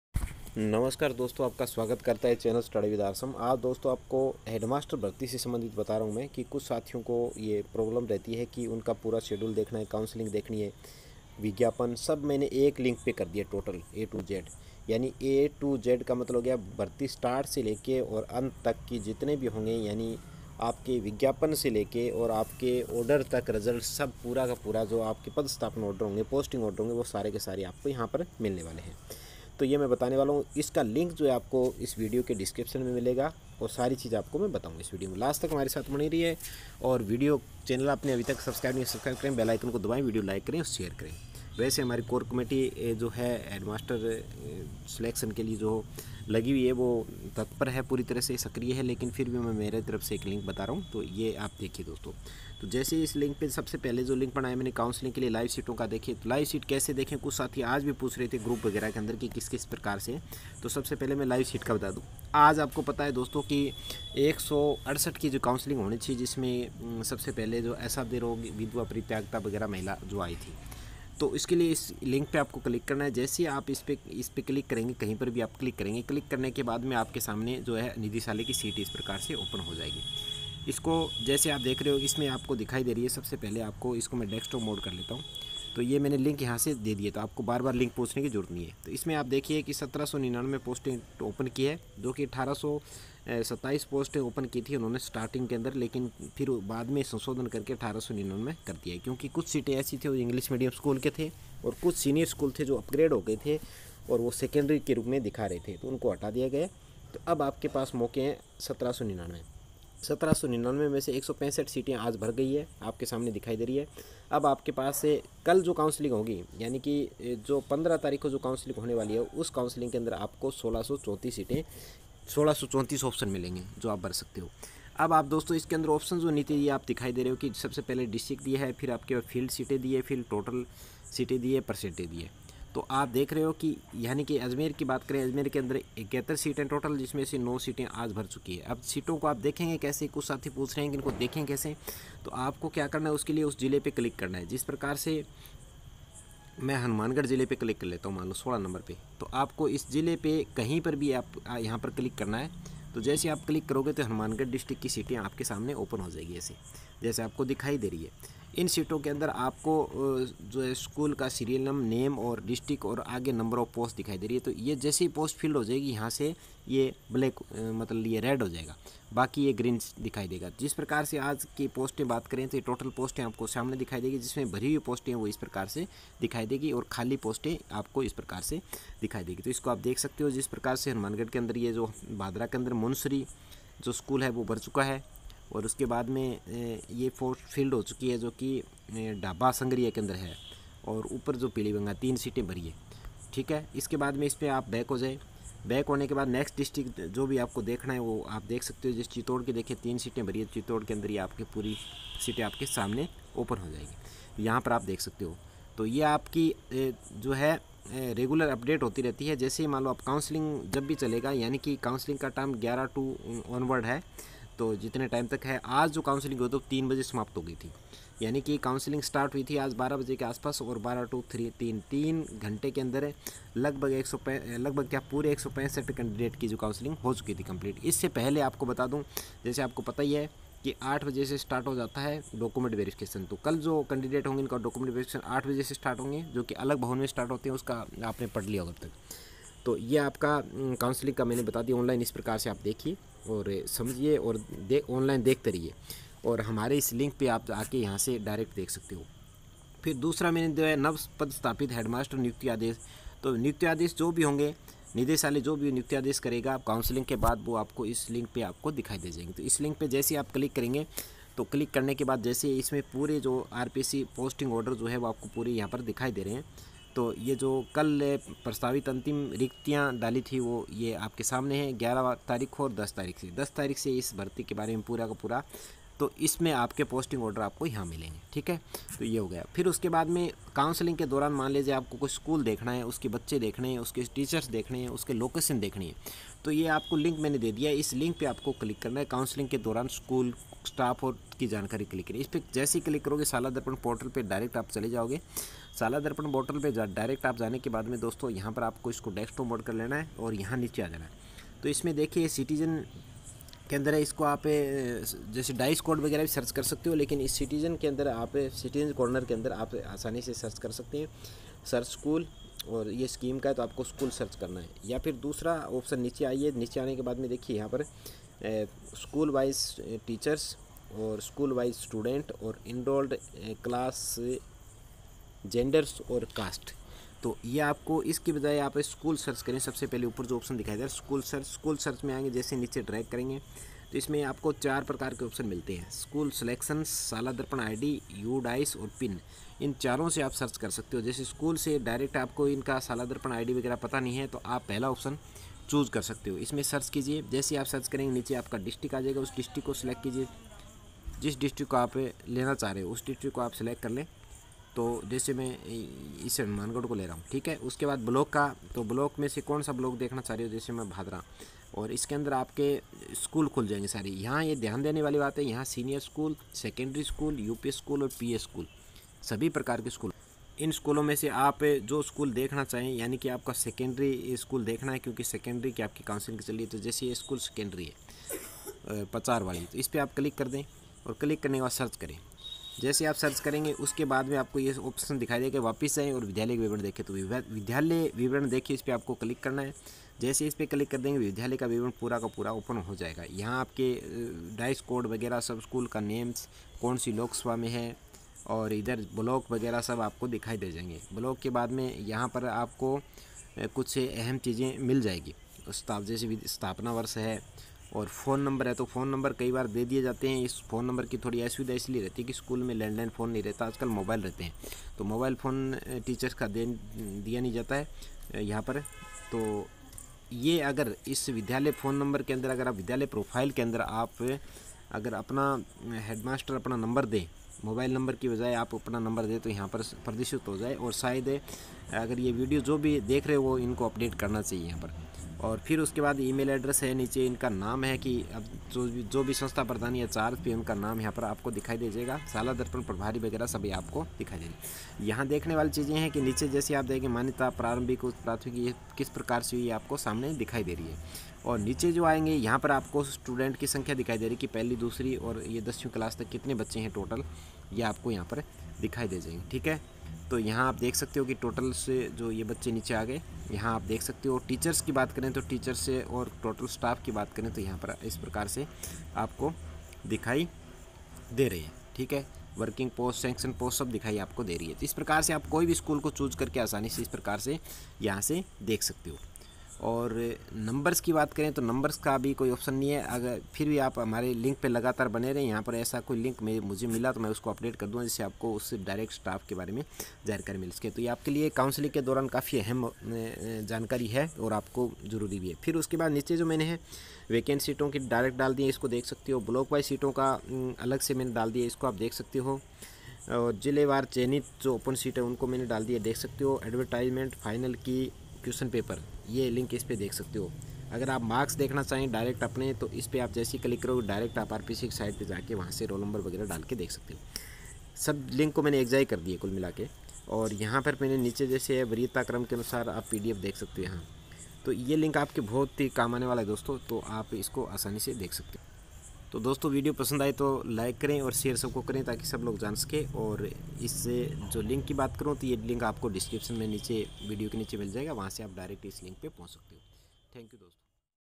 हैडमास्टर भर्ती 2018 आनलाईन काउंसलिंग में बहुत काम आने वाला
rpsc headmaster_counselling_posting Headmaster Recruitment 2018.

नमस्कार नवचयनित हैडमास्टर साथियों।
पुरी हैडमास्टर भर्ती विज्ञापन से लेकर पदस्थापन तक सभी एक ही लिंक एक ही प्लेटफार्म पर उपलब्ध करवाया जा रहा है, ताकि आपको बार2 किसी से पूछने की जरूरत ना पड़े, तुरंत आपको सब एक जगह ही मिल जाये और रिक्त सीटों का विवरण और काउंसलिंग लाईव 14-18 अक्टूबर को होने वाले और भविष्य में भी इस भर्ती से सम्बन्धित सभी प्रकार के कागजात इसी लिंक पर मिलेंगे।
Counselling live updation start🖕🖕
सबकुछ एक ही लिंक पर देखिए
धन्यवाद 🙏🙏